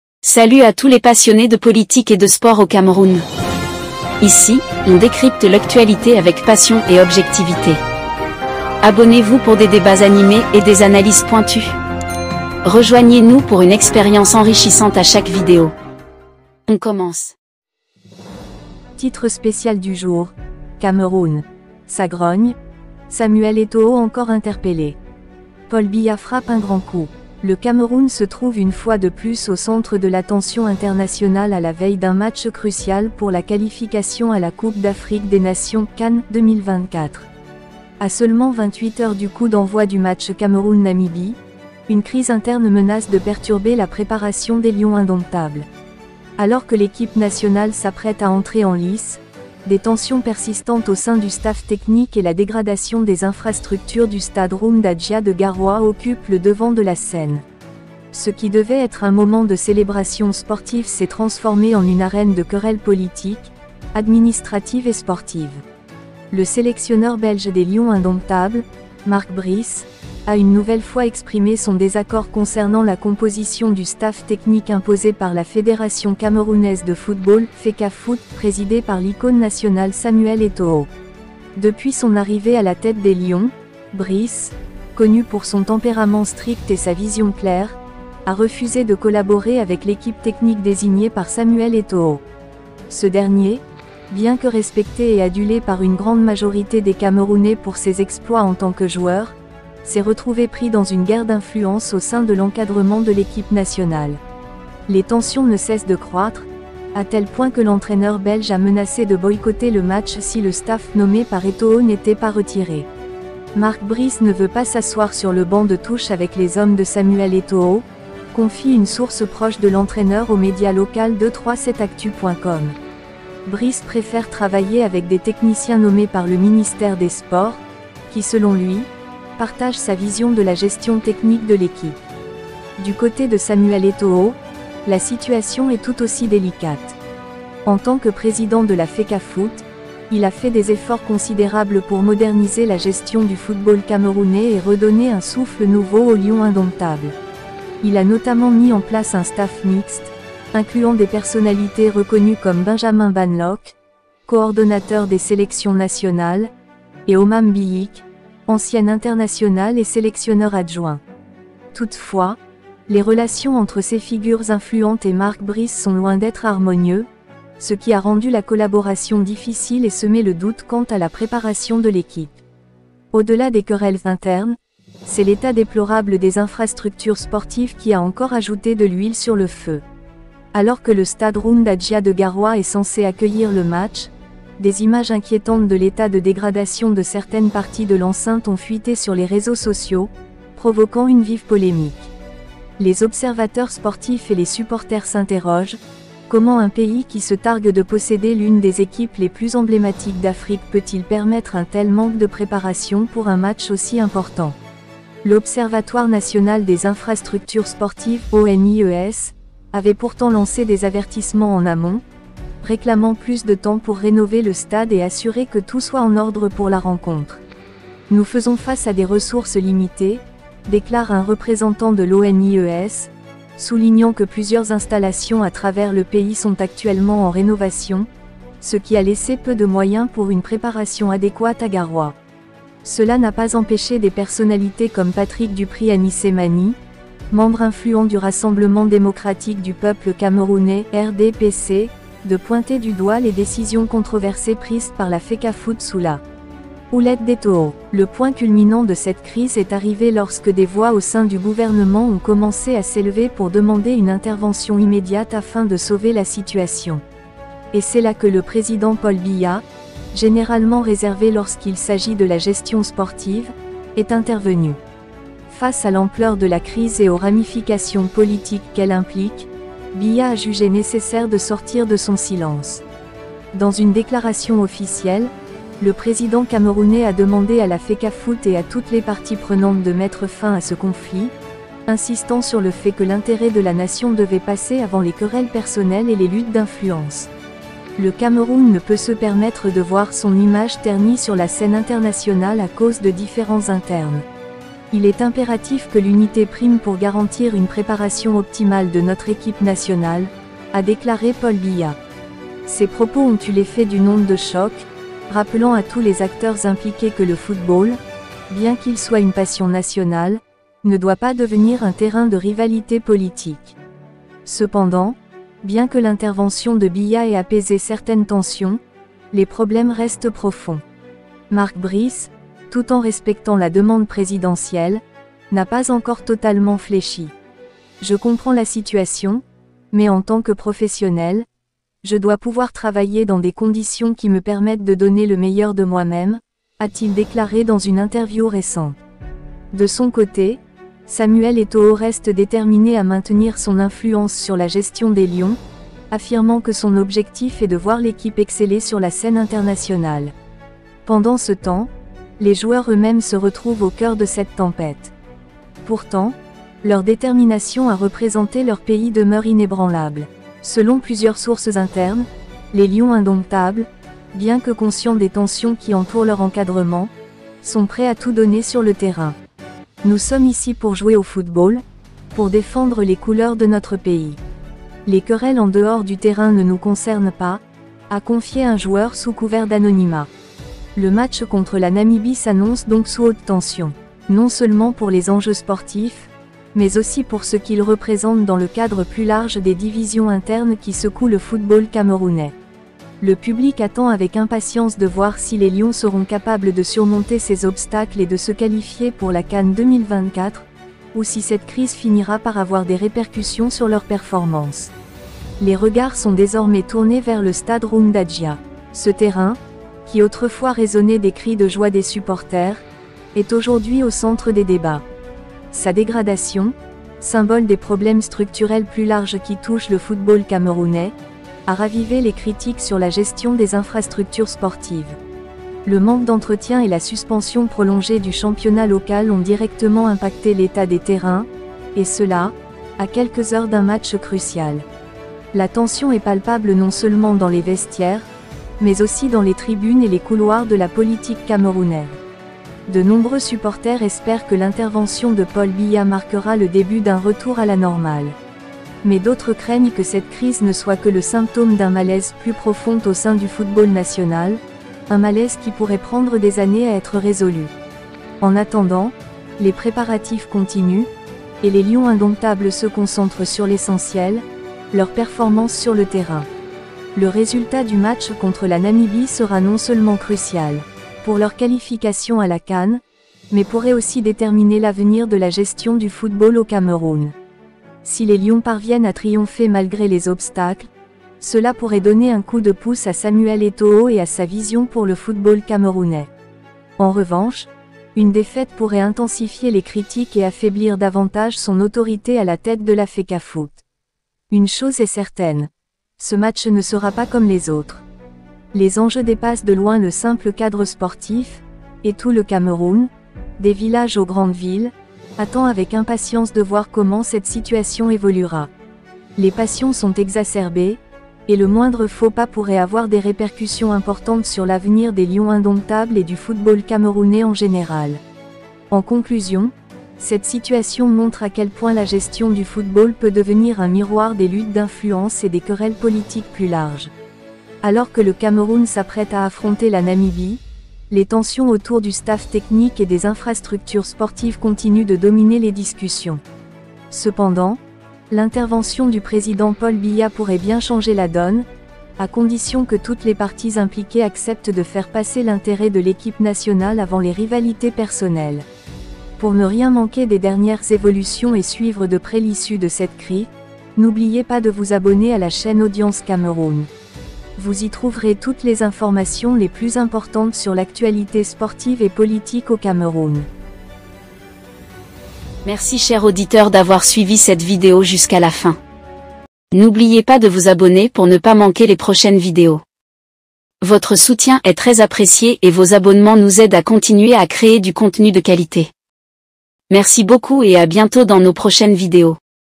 Ça tombe mal, Ça grogne, Samuel Eto’o encore interpellé !. Paul a finalement tranché.
Le Cameroun se trouve une fois de plus au centre de l’attention internationale à la veille d’un match crucial pour la qualification à la Coupe d’Afrique des Nations (CAN) 2024. À seulement 28 heures du coup d’envoi du match Cameroun-Namibie, une crise interne menace de perturber la préparation des Lions Indomptables. Alors que l’équipe nationale s’apprête à entrer en lice, des tensions persistantes au sein du staff technique et la dégradation des infrastructures du stade Roumde Adjia de Garoua occupent le devant de la scène. Ce qui devait être un moment de célébration sportive s’est transformé en une arène de querelles politiques, administratives et sportives.